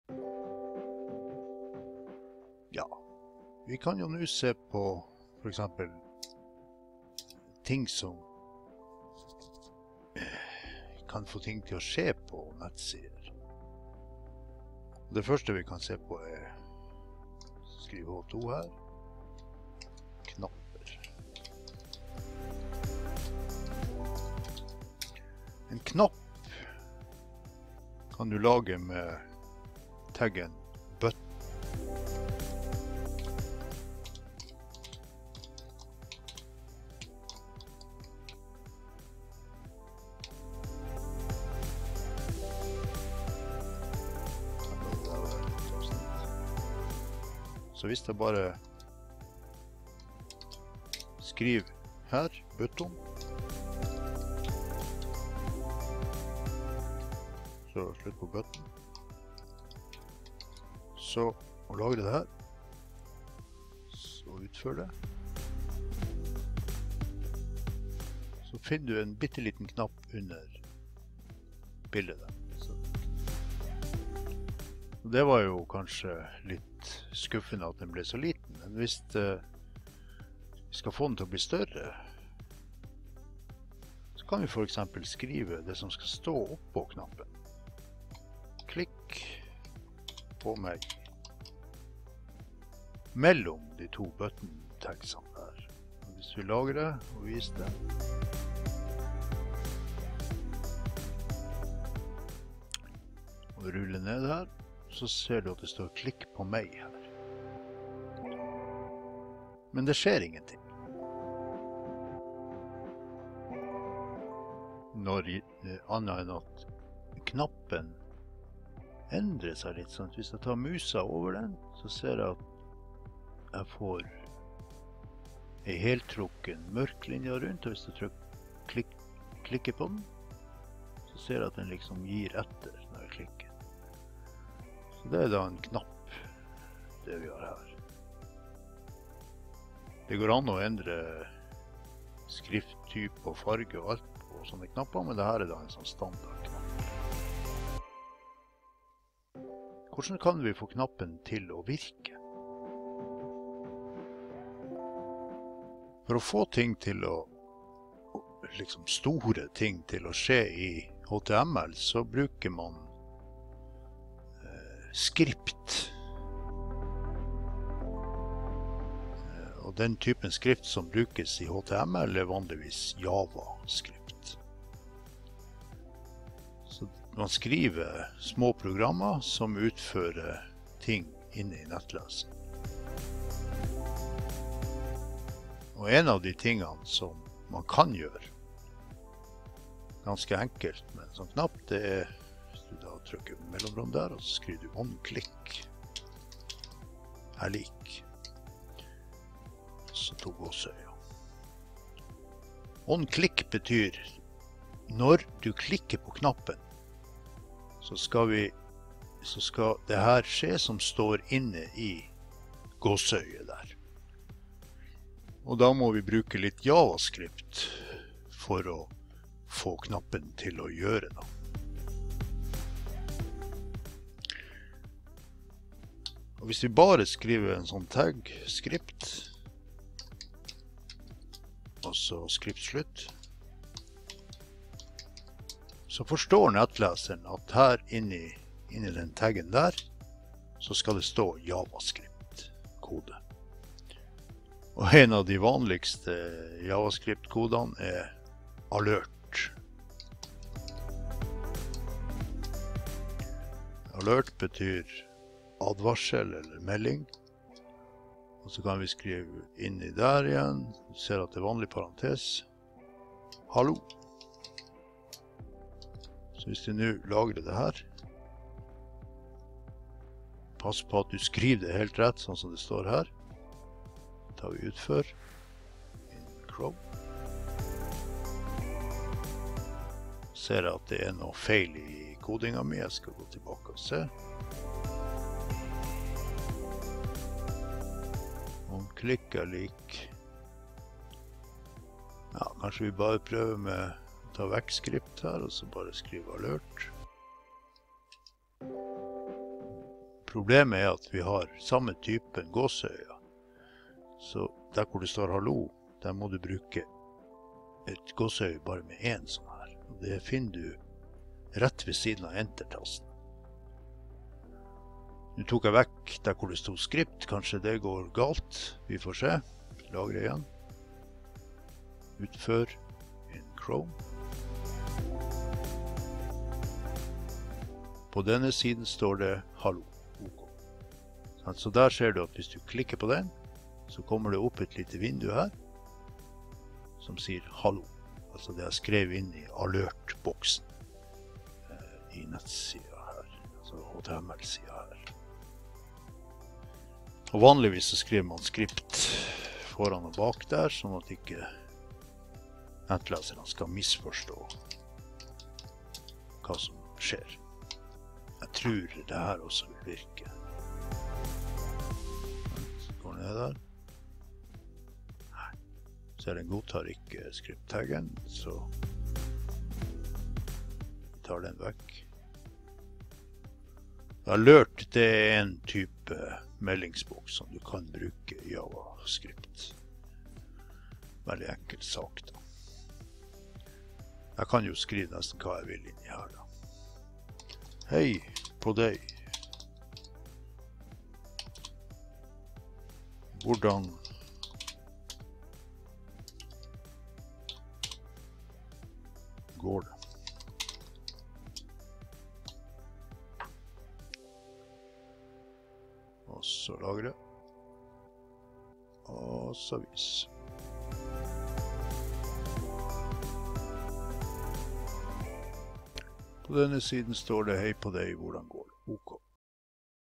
13 knapp og alert boks
Knapper med button taggen, en alert-boks koblet til knappen ved å bruke onclick= metoden.